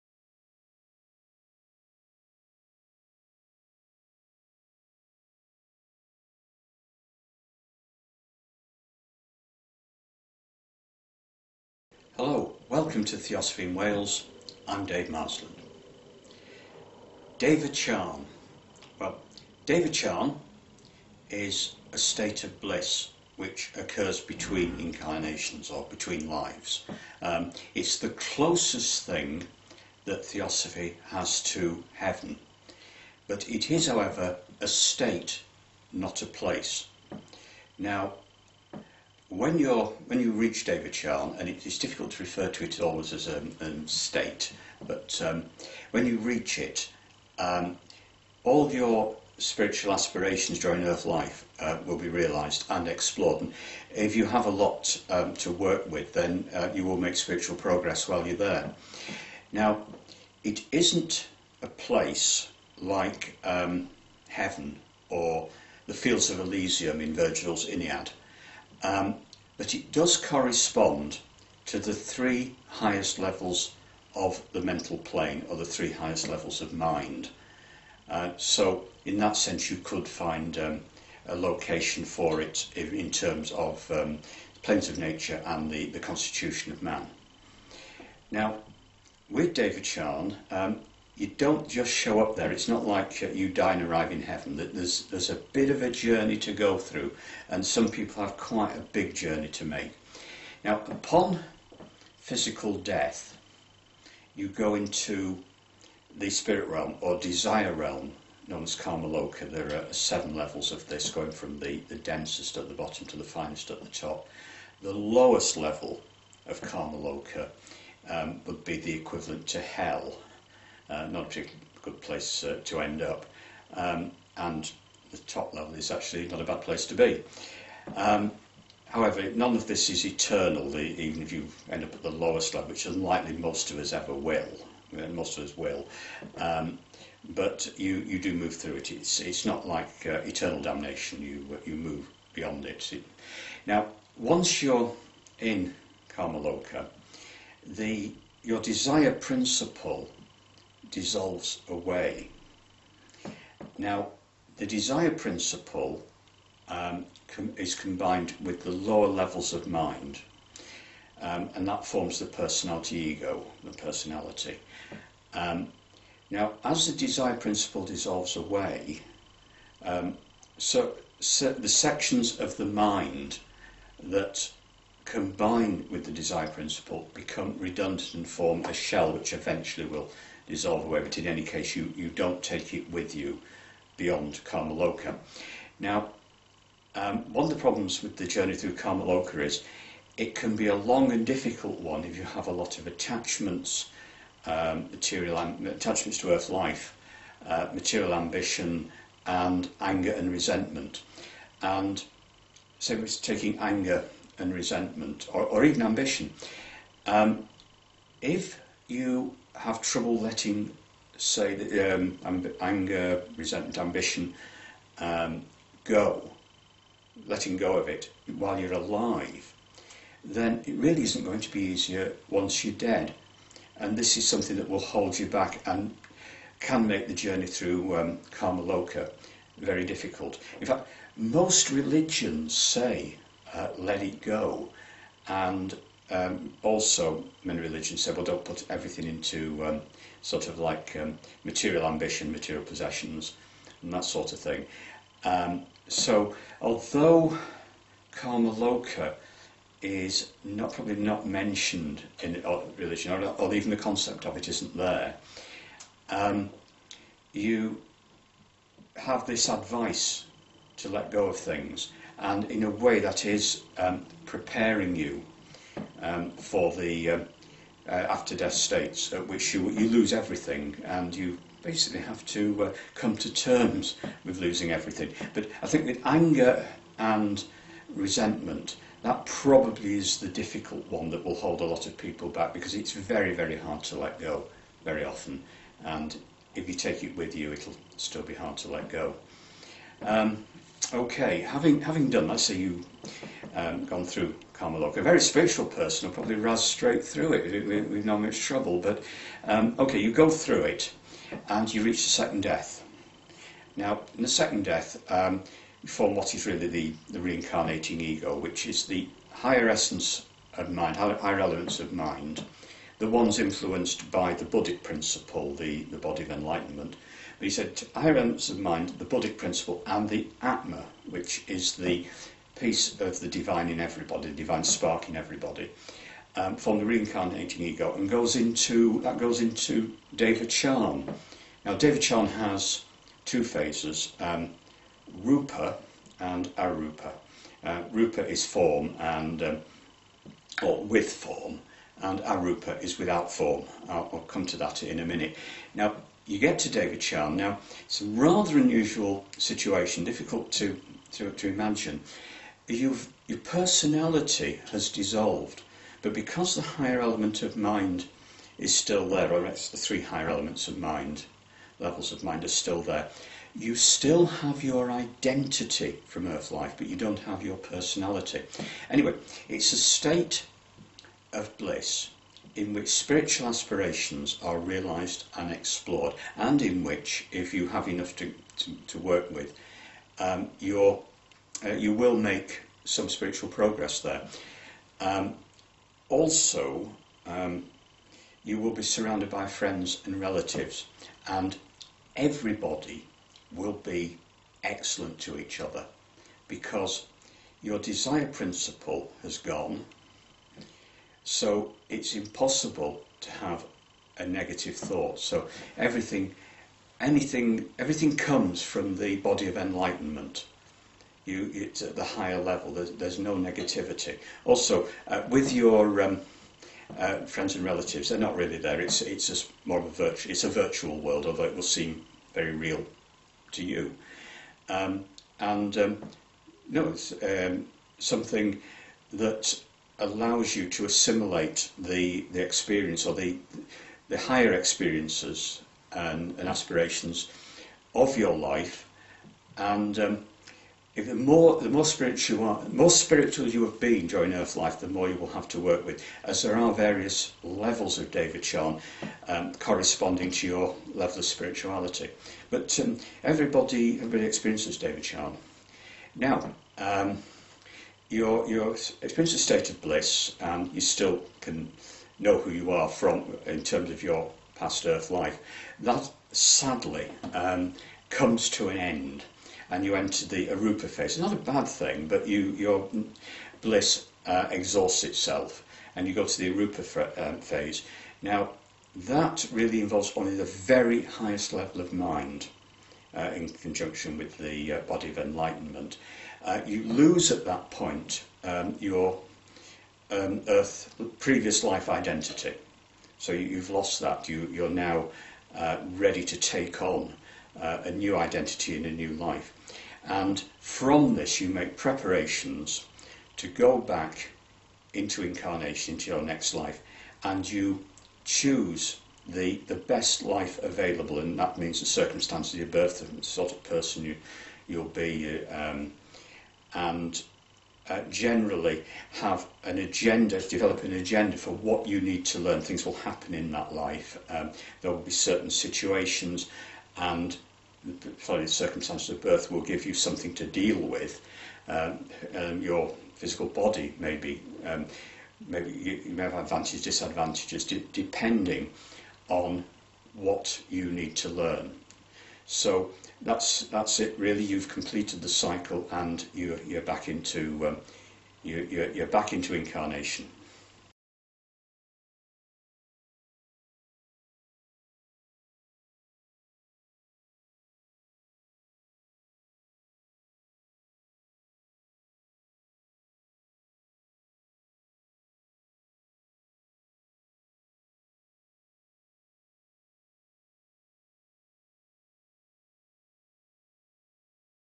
Devachan
Devachan presented by Dave Marsland of Cardiff Theosophical Society